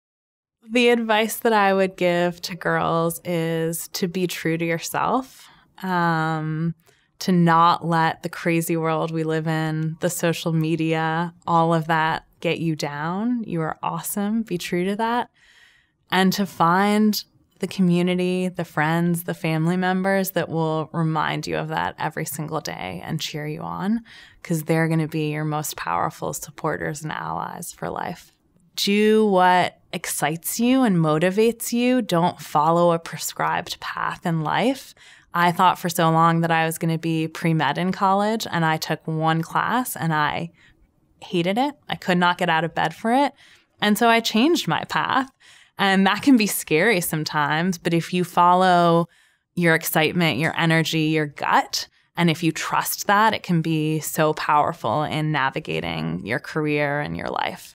Advice on Being True to Yourself | Management Consultant Erin Barringer | Career Girls
Erin Barringer is a Management Consultant, and Partner at Dalberg Advisors.